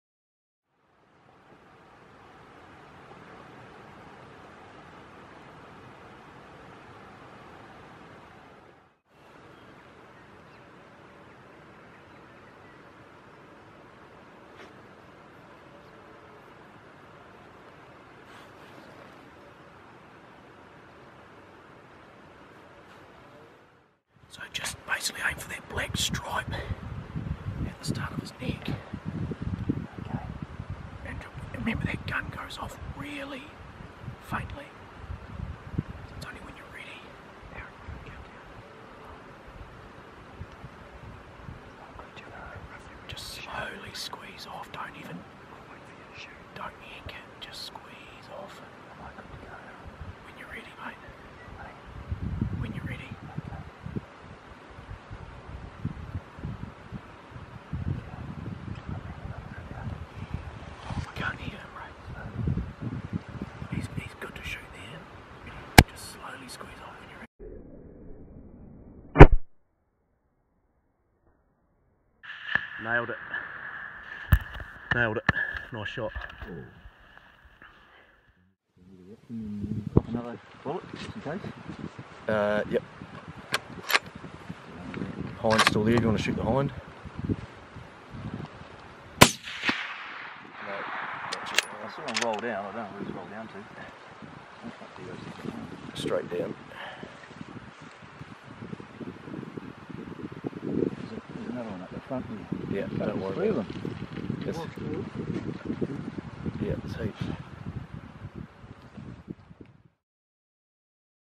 Imre Stag #3
The roar was late this year and Imre made the most of the stags following the hinds. He takes his third animal in perfect circumstances shooting from an elevated position at 140 yards. the shot was true and another trophy is taken